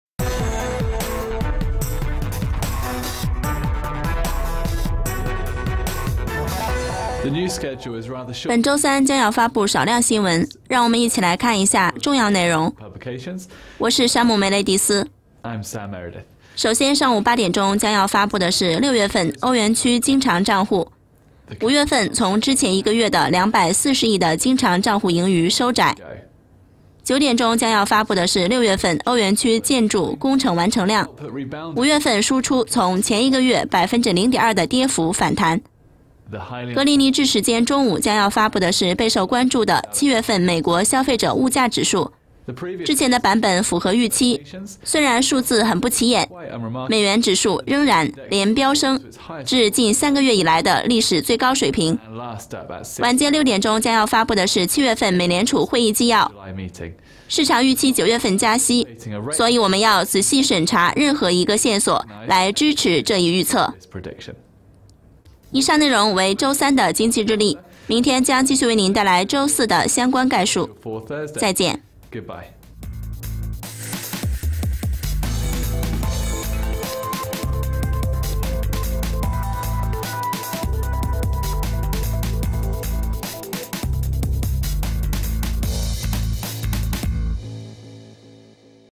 经济日历
本周三将要发布少量新闻，让我们一起来看一下重要内容，我是山姆.梅雷迪斯。